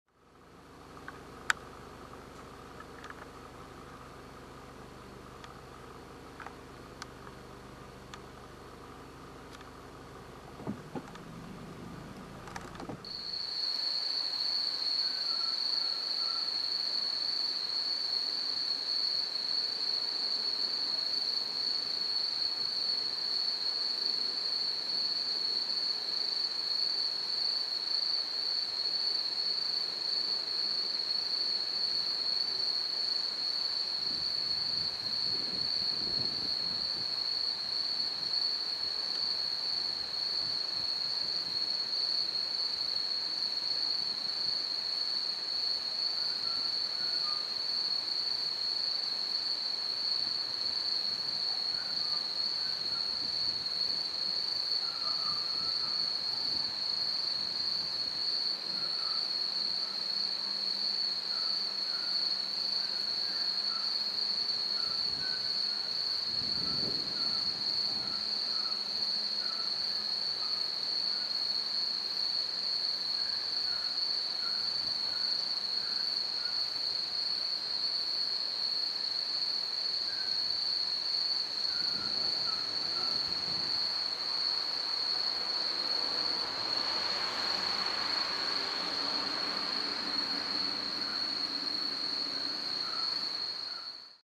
白タヌキがやって来た
夕方家に帰ると駐車場の端に白タヌキが来ていました。カメラを据えて見たらキョロキョロしながら出てくる白タヌキを撮影することが出来ました。家の前で見たのは久しぶりです。